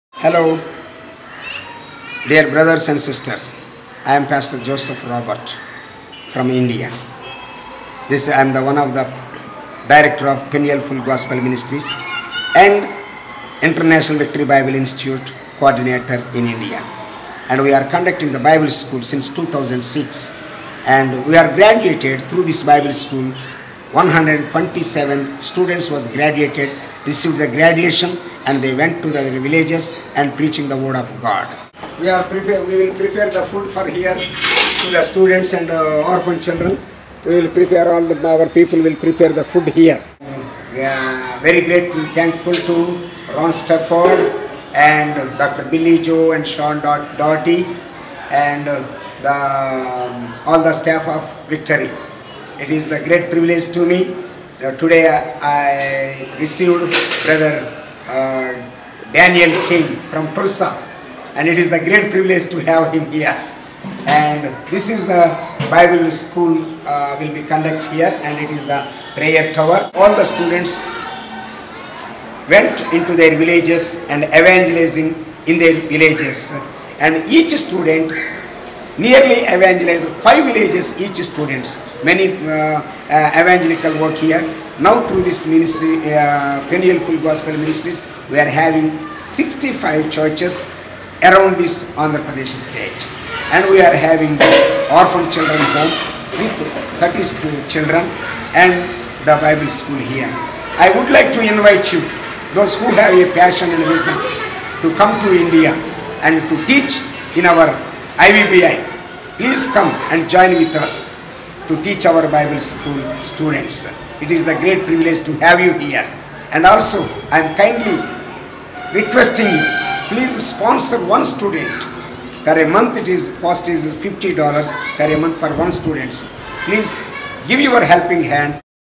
IVBI - India - Joseph Robert
International Victory Bible Institutes are all over the world. Here is a highlight from a school in India.